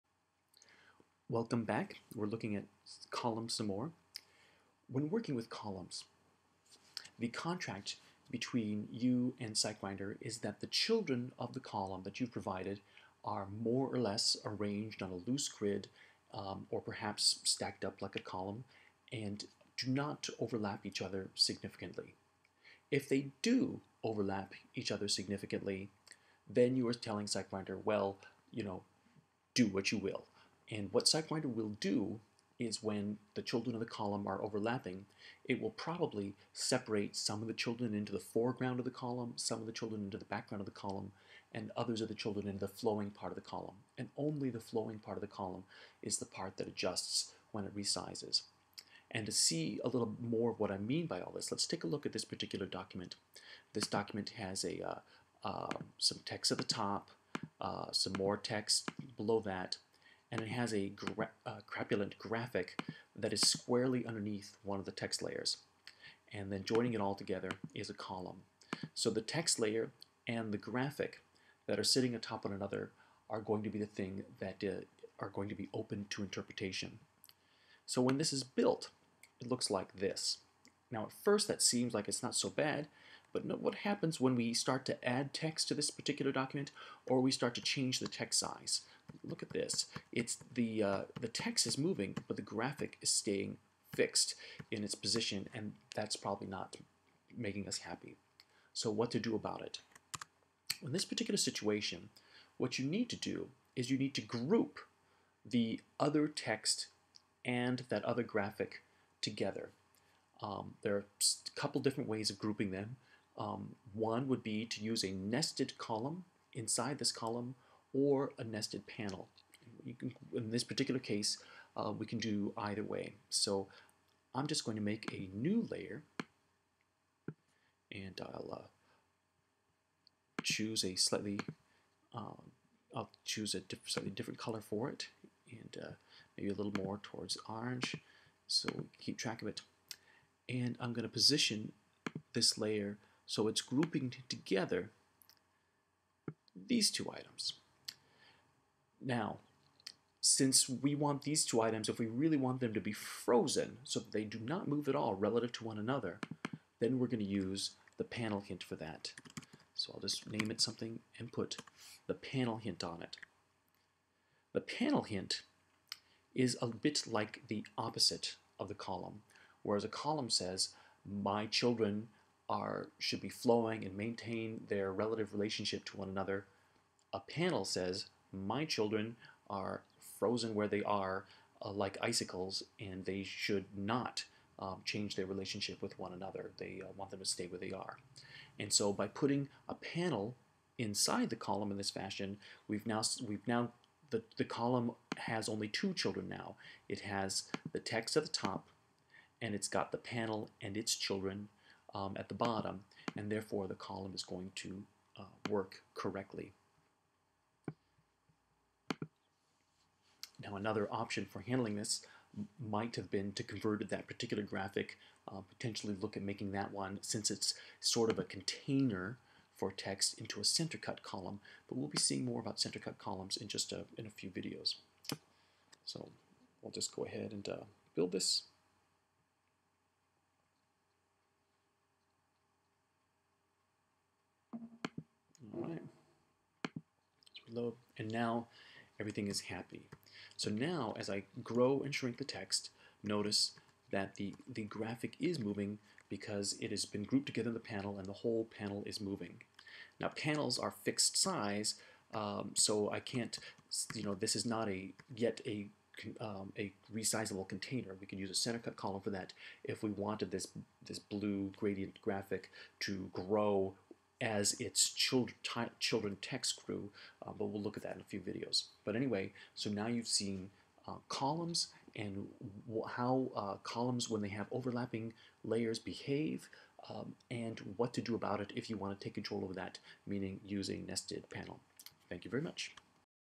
19-Application of the -Column Hint & Panels - Photoshop to HTML Websites with SG3
- This video will teach you how to group elements inside of a -column in order to maintain the position of the elements relative to each other.  With SiteGrinder you can create a website in Photoshop and then let SiteGrinder convert your web design from PSD to HTML.